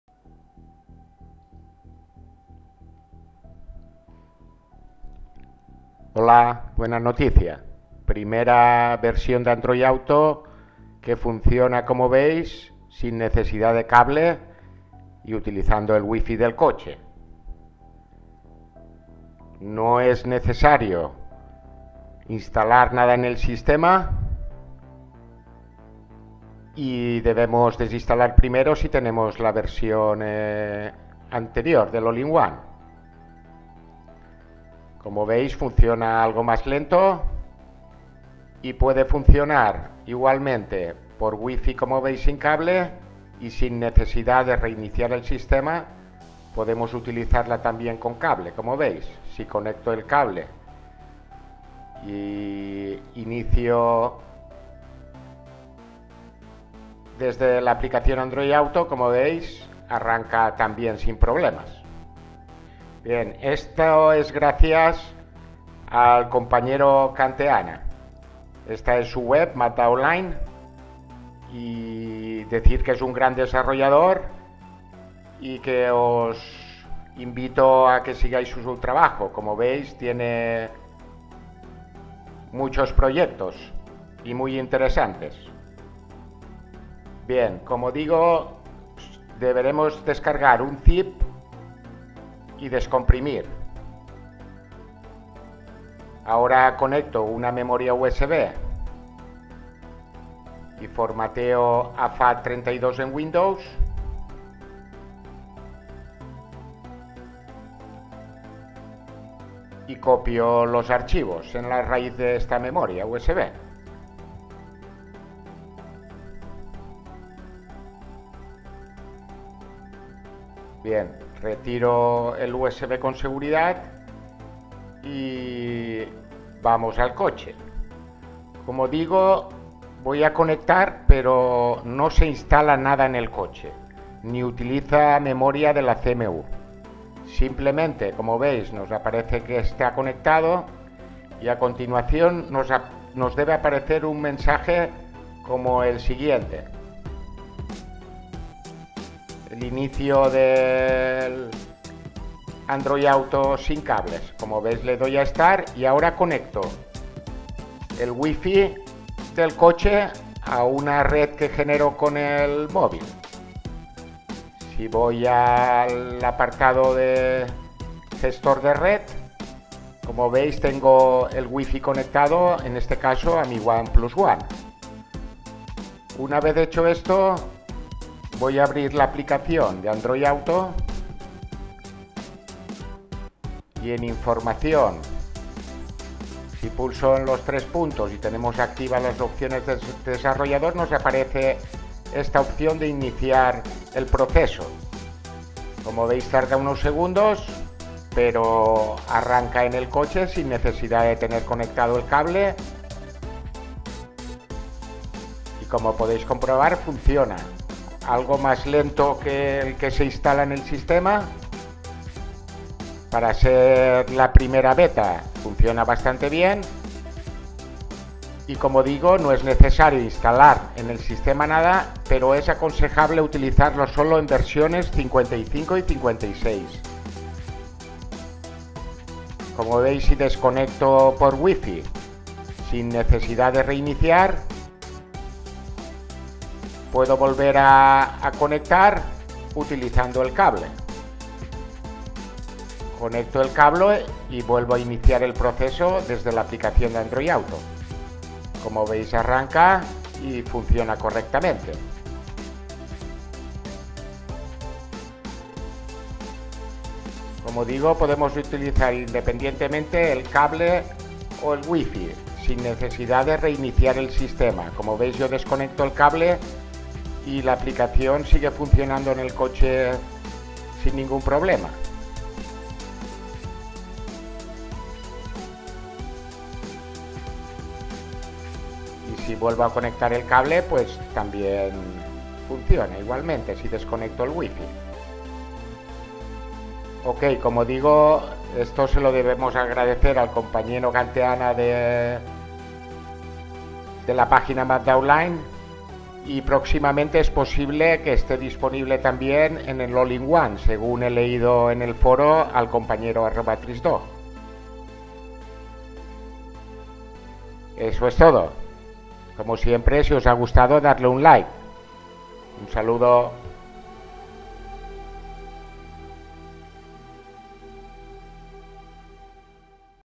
ANDROID AUTO WIFI MAZDA CONNECT 2018 (Without Installation) WIRELESS
THANKS @Khantaena
Run headunit-wireless on V55 and V56.You can test without risk.
Utilice headunit-wireless en V55 y V56. Puede probar sin riesgo.